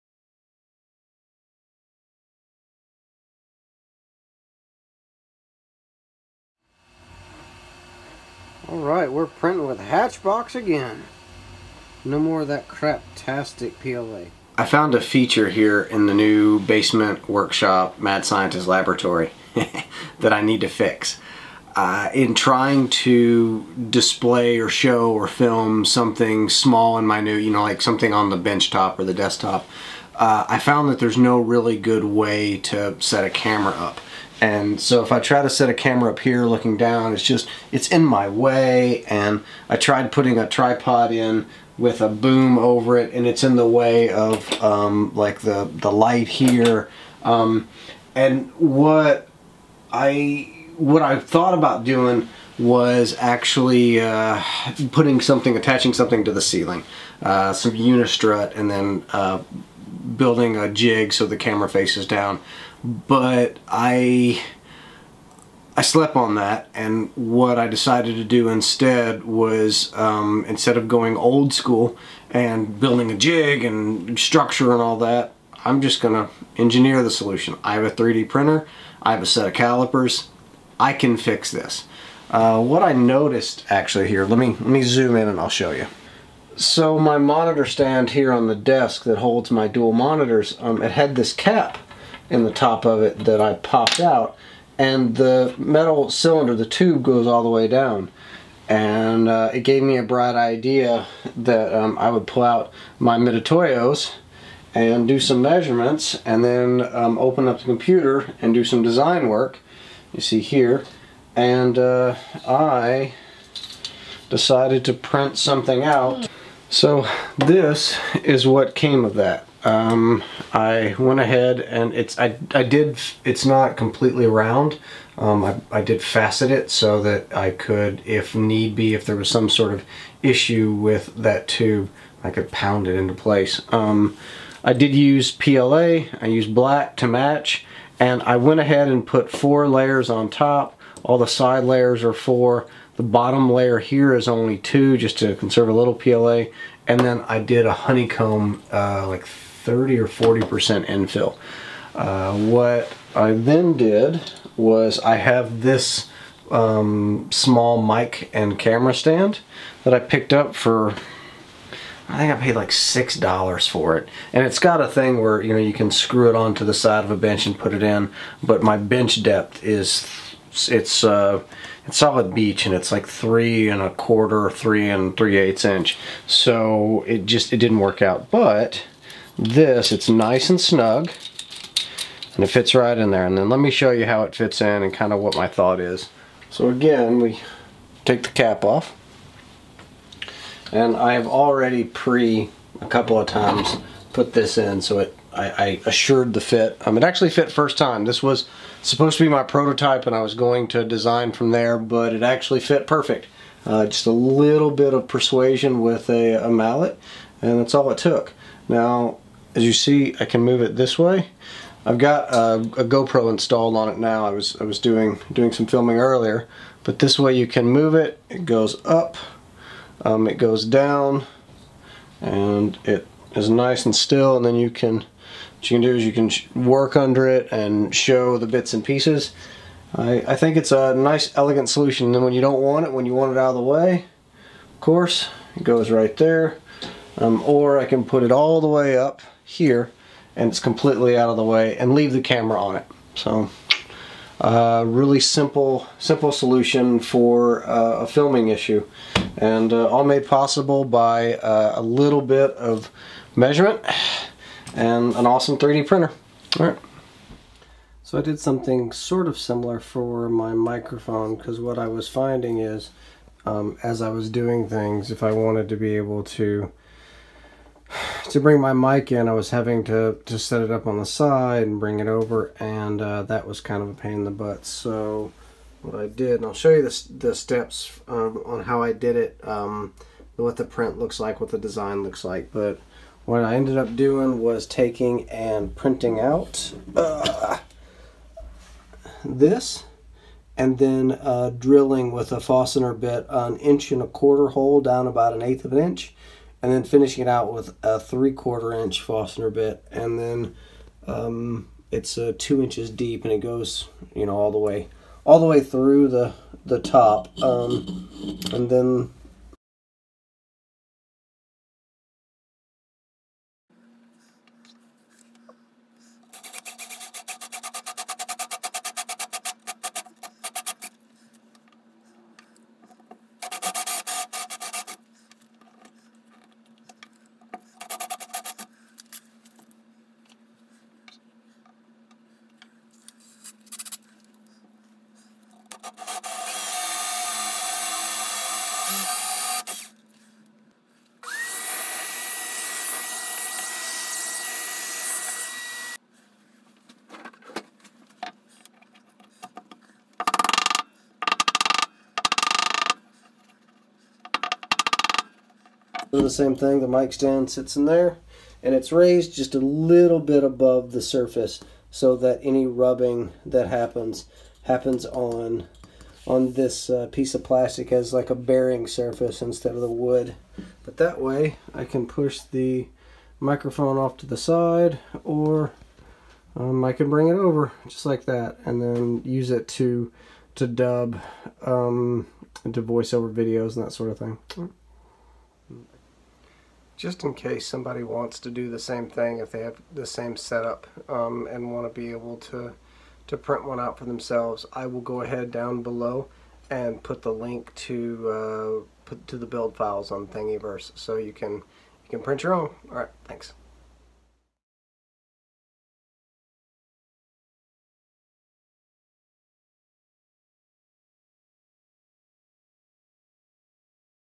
Workbench Camera and Microphone Mounts
I needed a solution for overhead filming on my bench top.  I was going to do a ceiling mount, but decided on a better approach that did not require me installing unistrut to the house ceiling/floor structure.   I have a monitor stand on my bench/desk with a single 1-1/4” inside diameter structural metal tube.  I removed the plastic cap and printed a 2" long bushing with a .475" center hole to fit a cheap articulating cam or mic stand.

Since I was on a roll, I designed and printed out something similar for a surface mount stand.  I am using it for a microphone, but it will work for a camera arm as well, as long as the outside dimension of your stand mount post is no greater than .475 - .48”

Camera Stand:

Microphone Stand: